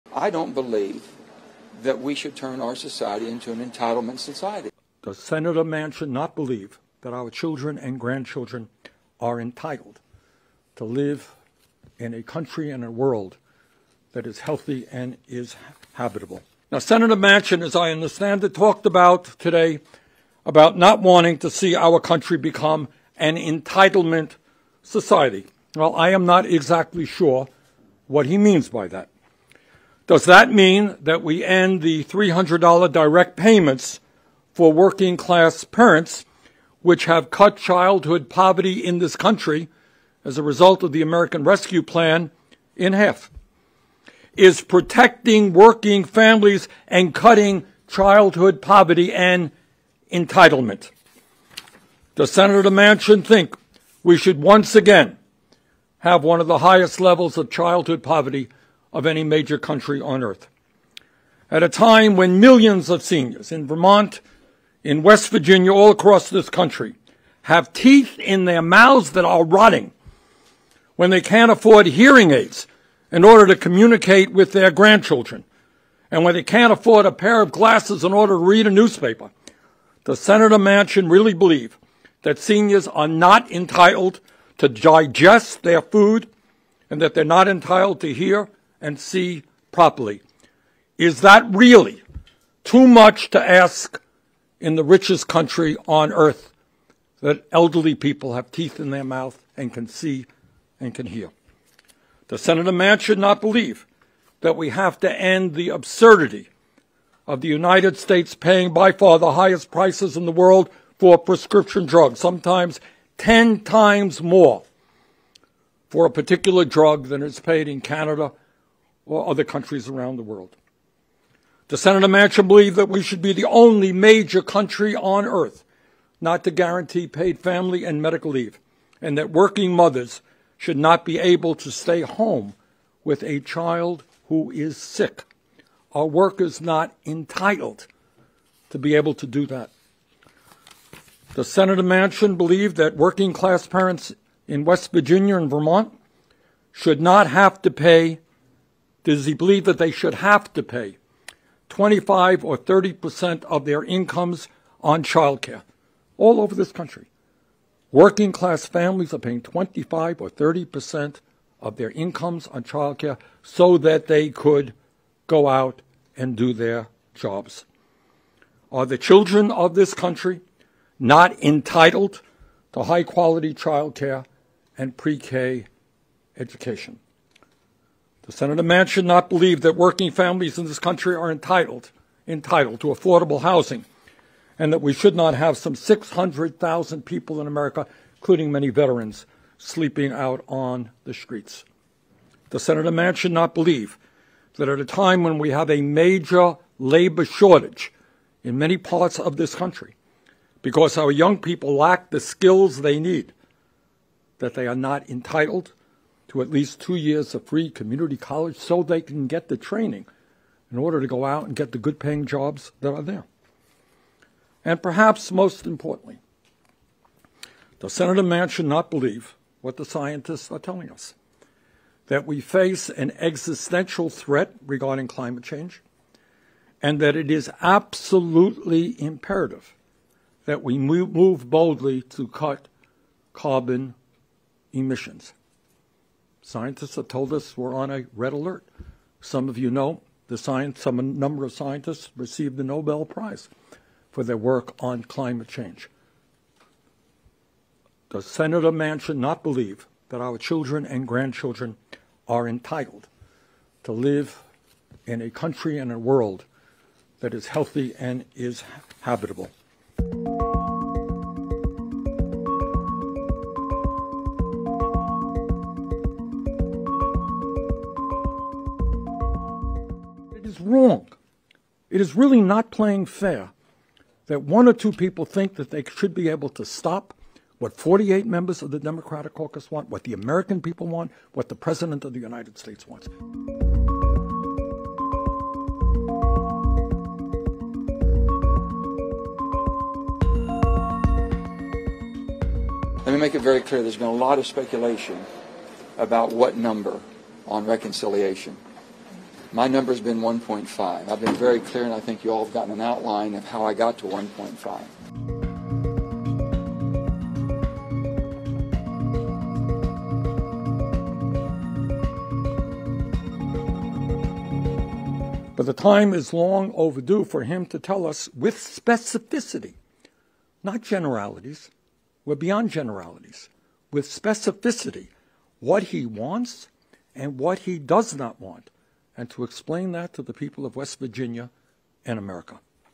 Bernie Sanders Calls Out Sen. Joe Manchin For Holding Dems Back
Bernie Sanders is calling out Sen. Joe Manchin for being one of only two Democrats holding the party back from passing major investments into care infrastructure.